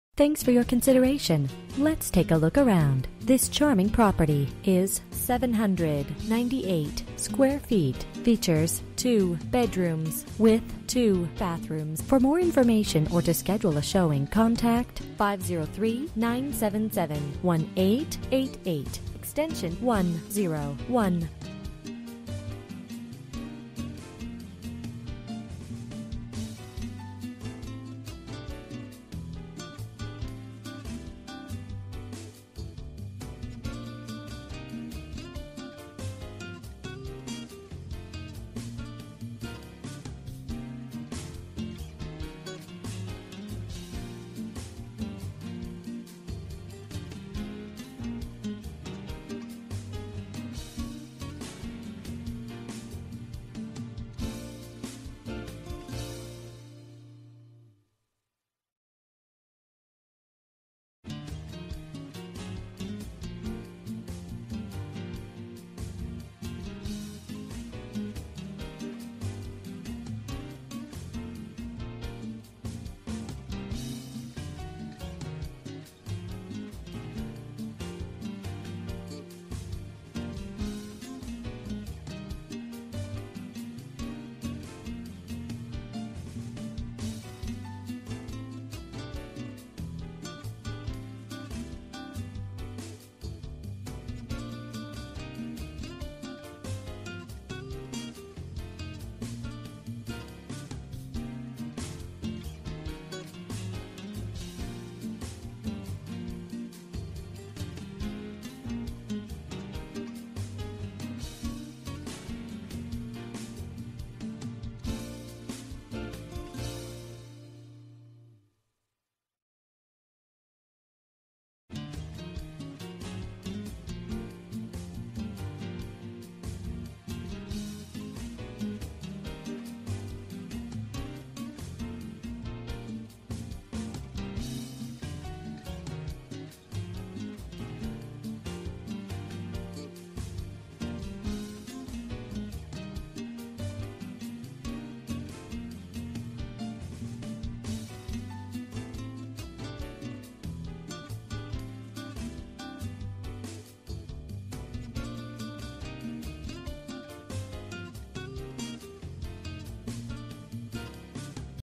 Property for sale - 2620 SW Moss Street, Portland, OR 97219
Bedrooms: 2
Bathrooms: 2 bathrooms
Square Feet: 798
Price: $299,000

MLS ID:

For more information about this property, please contact Patrick Connell at x101 or . You can also text 2651660 to 67299.

04/01/2015 12:39:02 pm

Last modified: